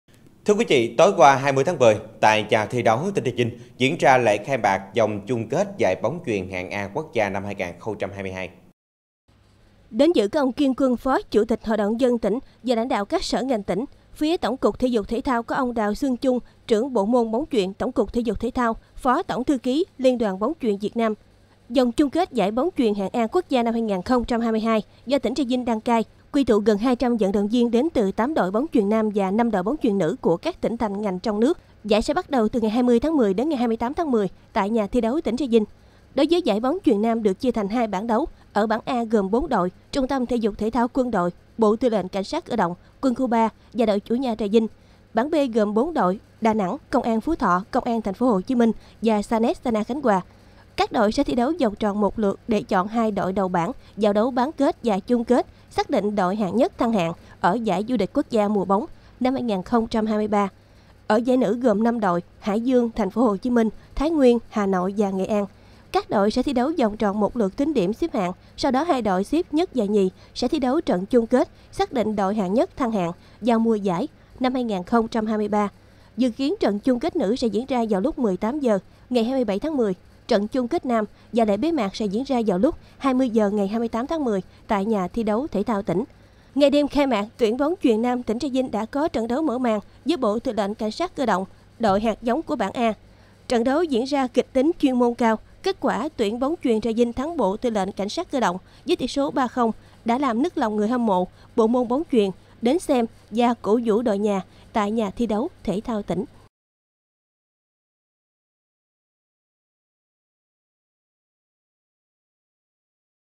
Khai mạc giải bóng chuyền hạng A quốc gia năm 2022
Tối 20/10, tại Nhà thi đấu tỉnh Trà Vinh diễn ra lễ khai mạc vòng chung kết giải bóng chuyền hạng A quốc gia năm 2022.
Đài Phát thanh và Truyền hình (PT&TH) Trà Vinh là cơ quan của Đảng bộ và chính quyền, đồng thời là diễn đàn của nhân dân tỉnh Trà Vinh.